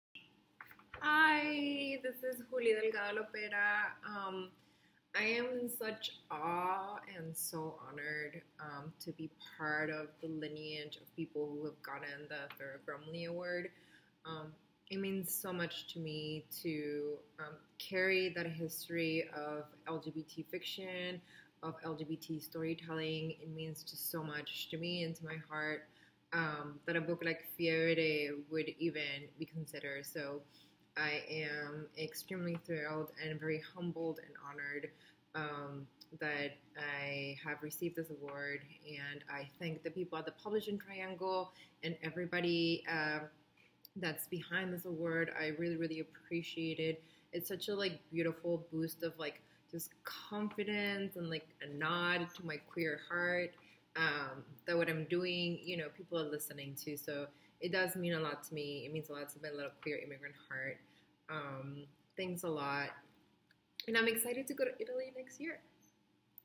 Juli Delgado Lopera Says Ferro-Grumley Award Is "Nod to My Queer Heart"
Fiebre Tropical, by Juli Delgado Lopera, is the winner of this year’s Ferro-Grumley Award for LGBTQ Fiction, as announced in May. In their thank-you remarks, the author says, "I'm so honored to be part of the lineage of people who have gotten the Ferro-Grumley Award."